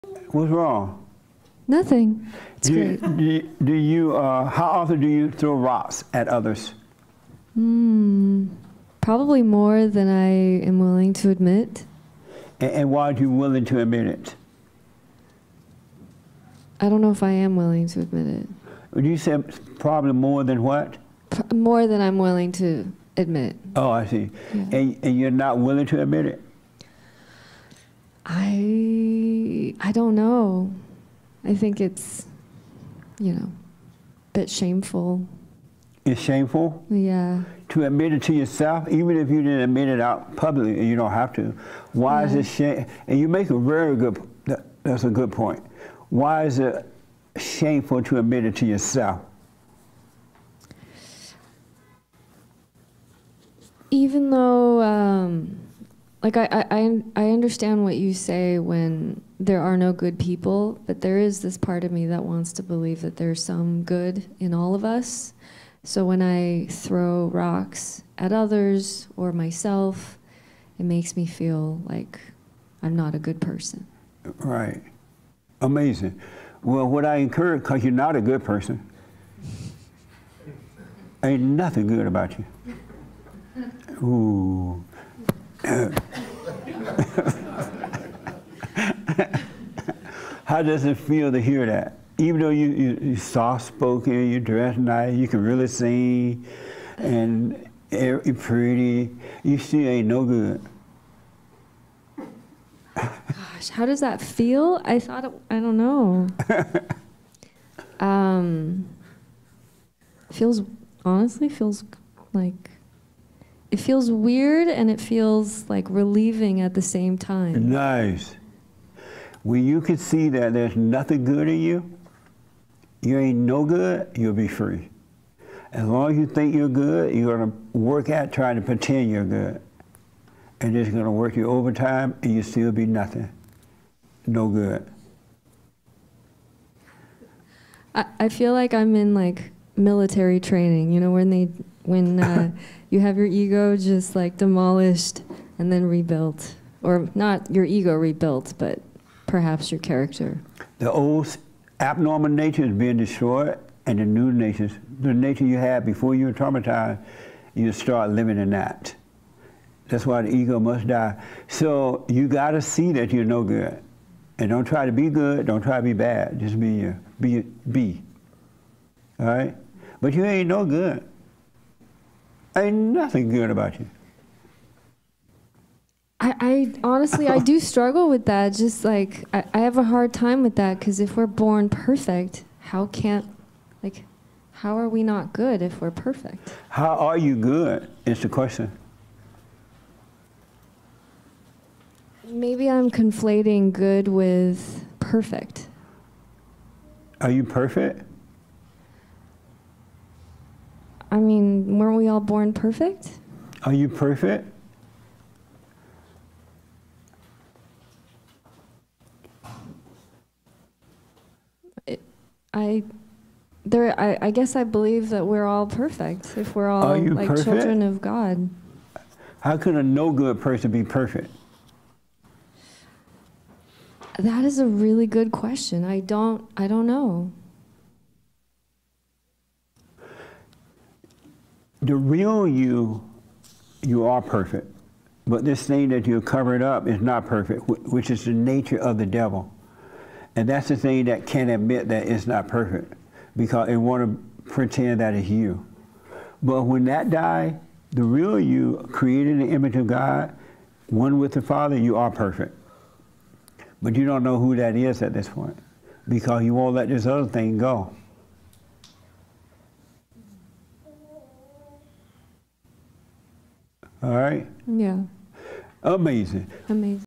It's a relief knowing you're no good.
JLP talks with a young lady Lijie on the Biblical Question: How often do you throw rocks at others? "Probably more than I'd like to admit." It feels like a shame, she says.

Church fellowship at BOND Sunday 11 AM PT (1 CT / 2 ET)